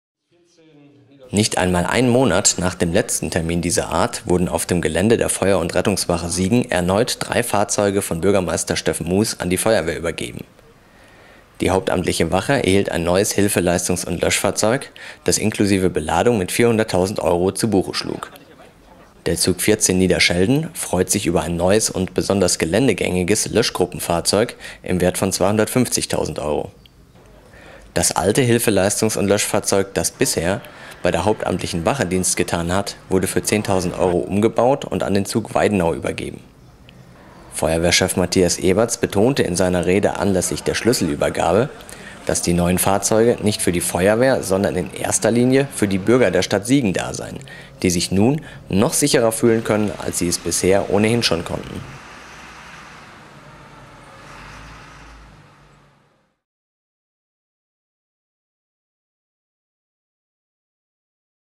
26.05.2011 Siegen: Neue Fahrzeuge
Nicht einmal einen Monat nach dem letzten Termin dieser Art wurden auf dem Gelände der Feuer- und Rettungswache Siegen erneut drei Fahrzeuge von Bürgermeister Steffen Mues an die Feuerwehr übergeben. Die hauptamtliche Wache erhielt ein neues Hilfeleistungs- und Löschfahrzeug HLF20, das inklusive Beladung mit 400.000 Euro zu Buche schlug. Der Zug 14 Niederschelden freut sich über ein neues und besonders geländegängiges Löschgruppenfahrzeug LF 10/6 im Wert von 250.000 Euro. Das alte Hilfeleistungs- und Löschfahrzeug LF 24, das bisher bei der hauptamtlichen Wache Dienst getan hat, wurde für 10.000 Euro umgebaut und an den Zug Weidenau übergeben. Feuerwehrchef Matthias Ebertz betonte in seiner Rede anlässlich der Schlüsselübergabe, dass die neuen Fahrzeuge nicht für die Feuerwehr sondern in erster Linie für die Bürger der Stadt Siegen da seien, die sich nun noch sicherer fühlen können, als sie es bisher ohnehin schon konnten.

Produziert für Siegen.TV
Kamera, Schnitt, Text, Sprecher: Kai A. Hortmann
Aufgenommen mit Sony NEX-VG10E.